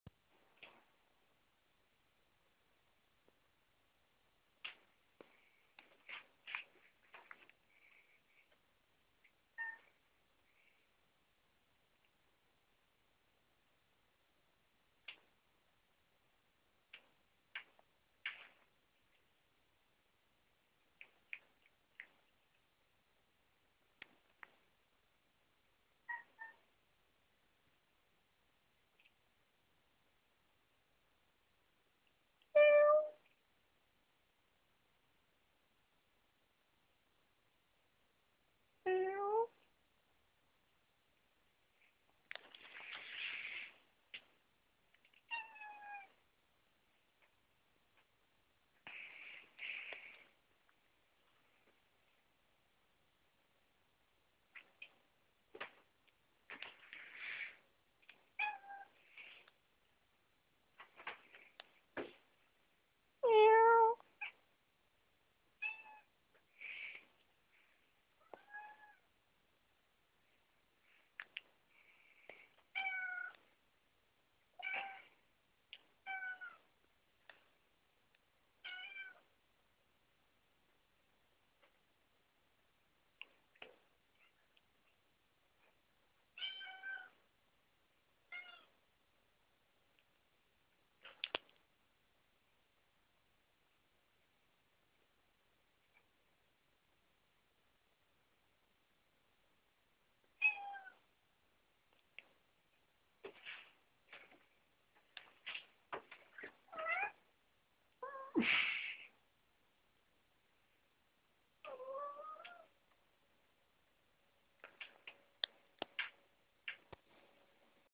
Cat door!
Rebecca's cat jumps on to a door while one of the other cats watches on. It looks like the other cat was hunting and Sparta (the black and white one) hand some sence to gain the high ground.